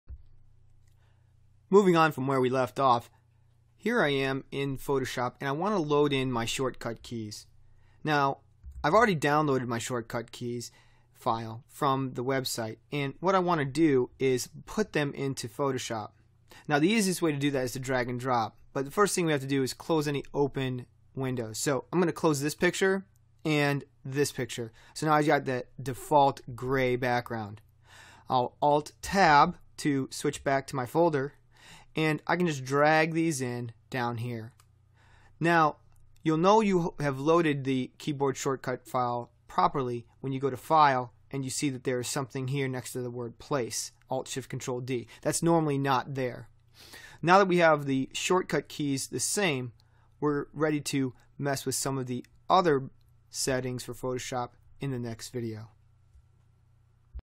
004 PS CS5 Keyboard Shortcuts
In this video, we show how to add our custom keyboard shortcuts.